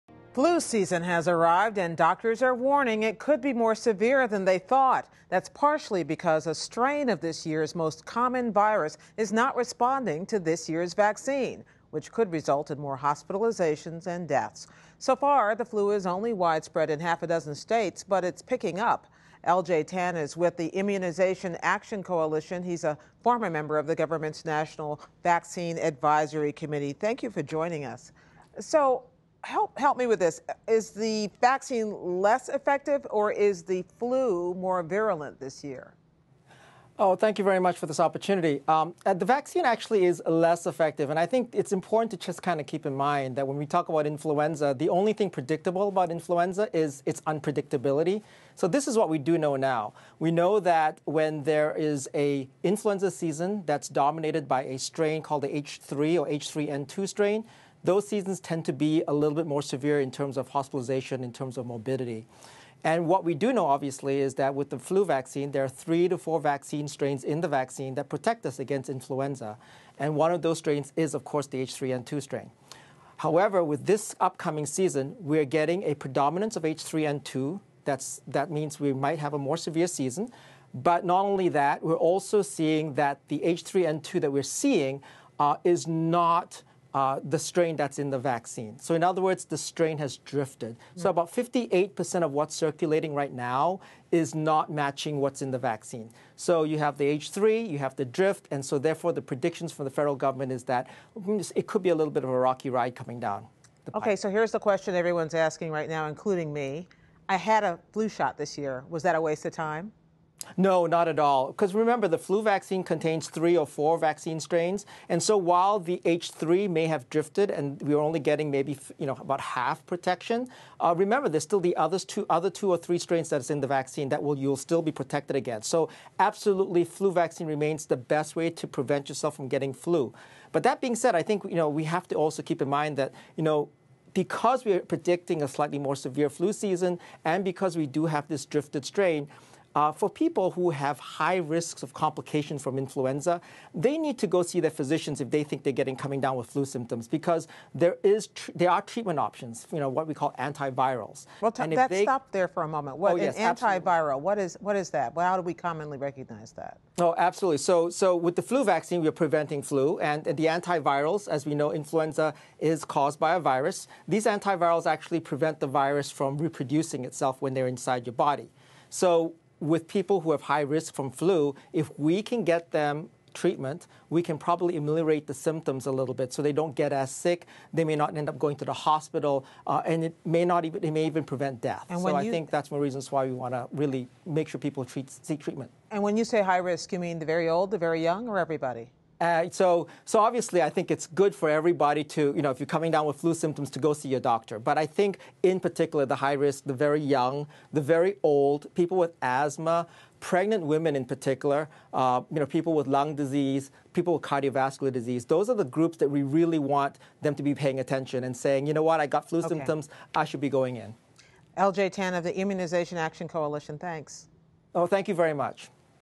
What you need to know about this year’s elevated flu threat
‘Tis the season to be sick in households across America, and this year’s flu shot may not save you. Doctors are warning that the season could be more severe than they thought because a strain of the virus is not responding to the vaccine. Gwen Ifill gets tips for how to prepare from L.J. Tan of the Immunization Action Coalition.